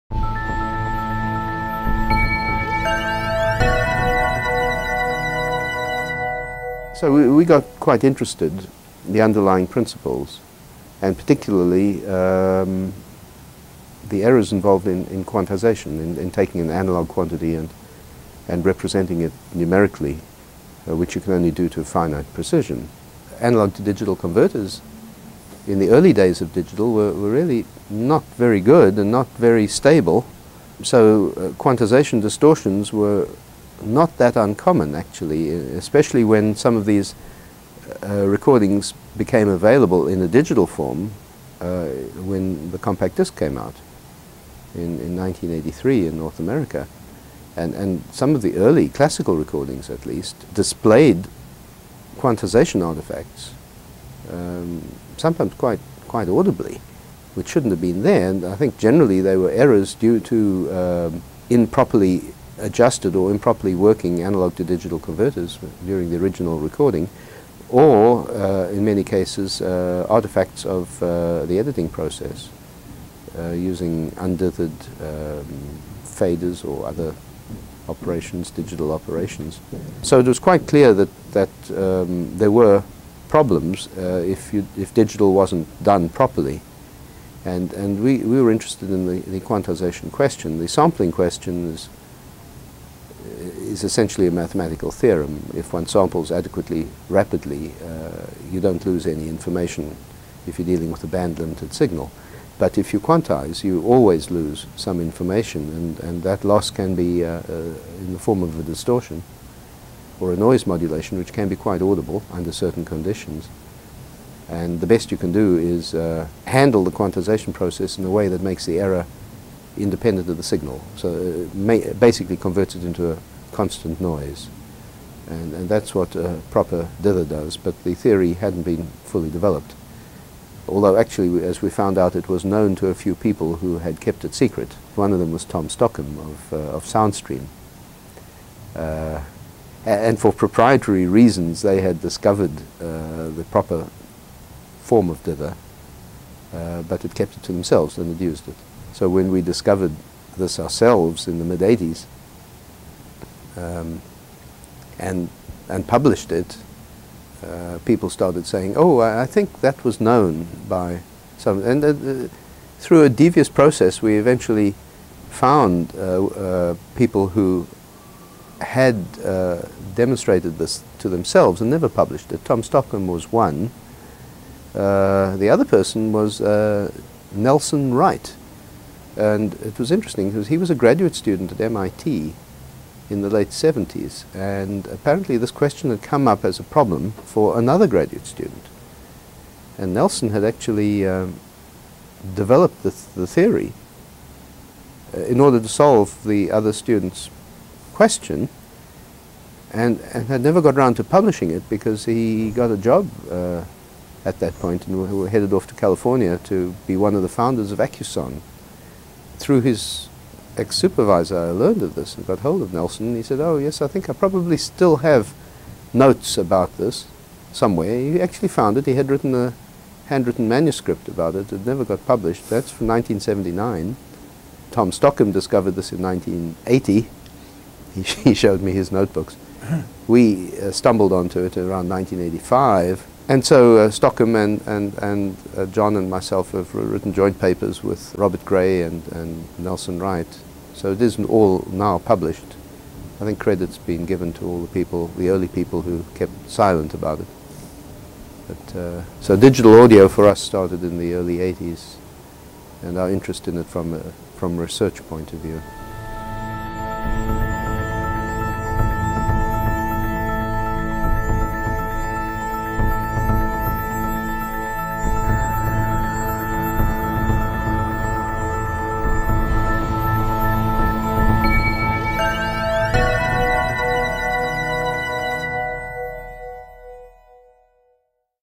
Stanley Lipshitz "Digital Audio" (Web 82)
AES Oral History - Stanley Lipshitz discusses the history of improvements in digital recording - for more go